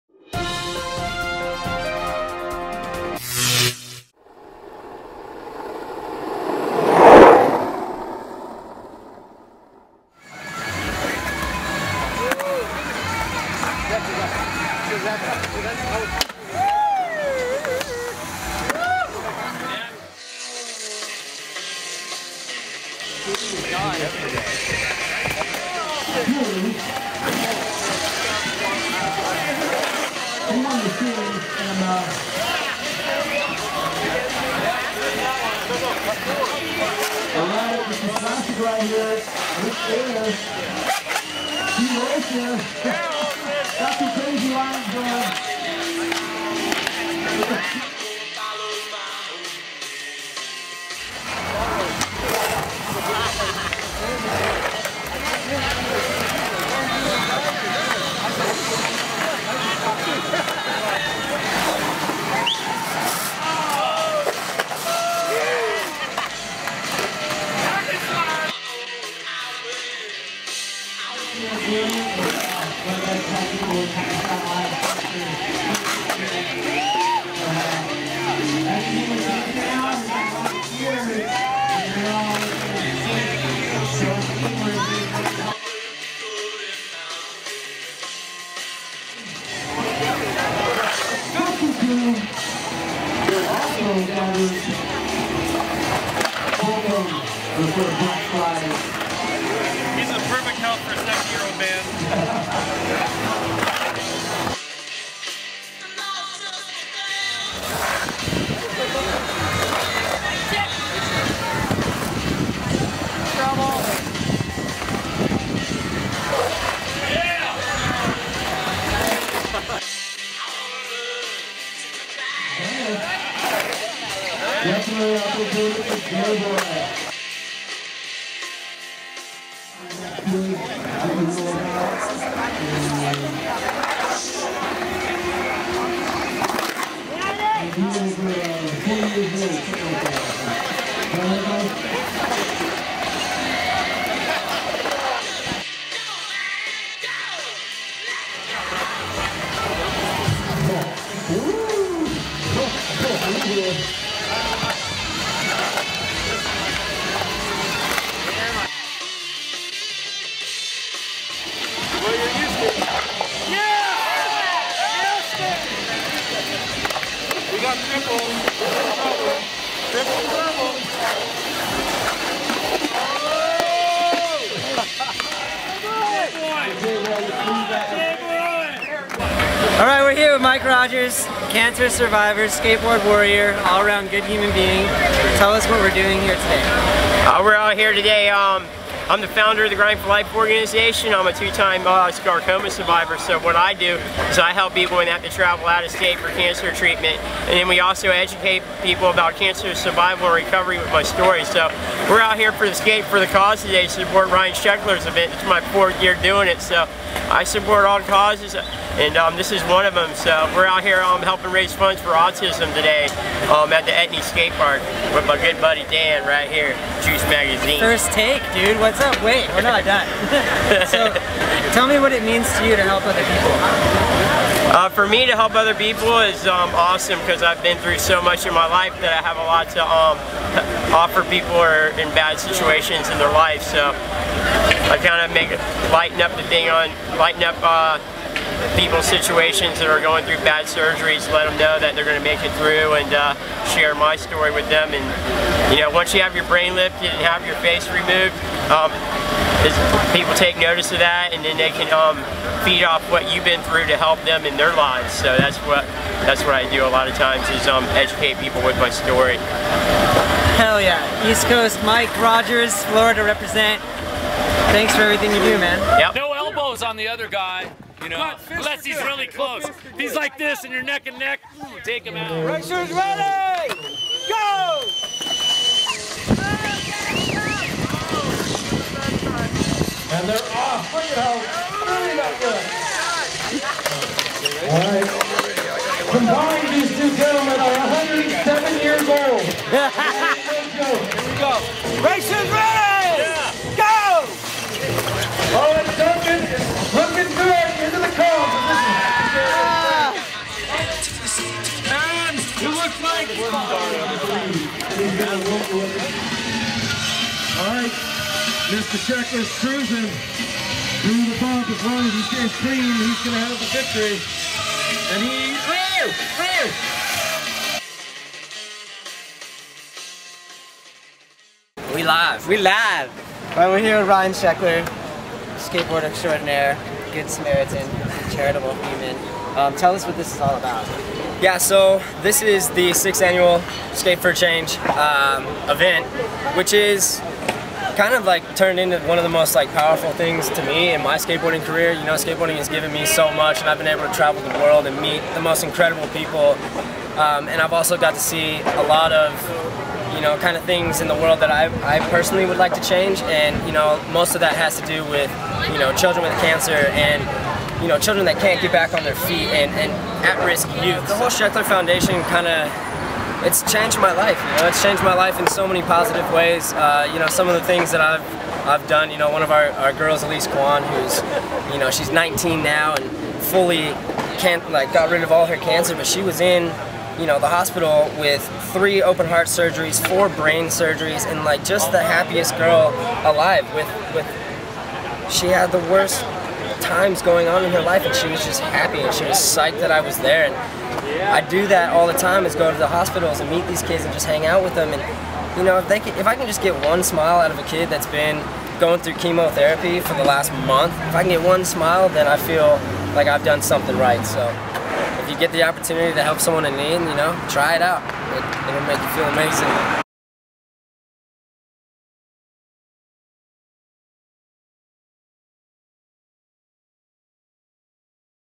Skate For A Cause 2015
6th Annual SKATE FOR A CAUSE, May 2, 2015 at etnies Skatepark – Lake Forest, California. Features interviews with Ryan Sheckler and Mike Rogers and skate clips from Charlie Blair, Steve Olson, Dave Hackett, Eddie Elguera, Dave Duncan, Eddie Havina, Jim Gray, Mike Rogers and Eddie Reategui. Filming and Photos by Dan Levy and Vans Davey.

Etnies is proud to present, the Sheckler Foundation’s 6th Annual Skate for a Cause, a family friendly event that combines a world class skate jam with a carnival. We have invited the top names in skateboarding to participate in this unique skate event that will benefit our Be The Change initiative.